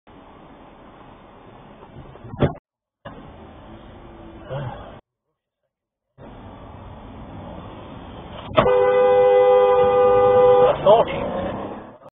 Yet another none existent right turn, turner. Mercedes P21 KLF. Trent Bridge, Nottingham 18.5.23
Usual suspects, same place, different day.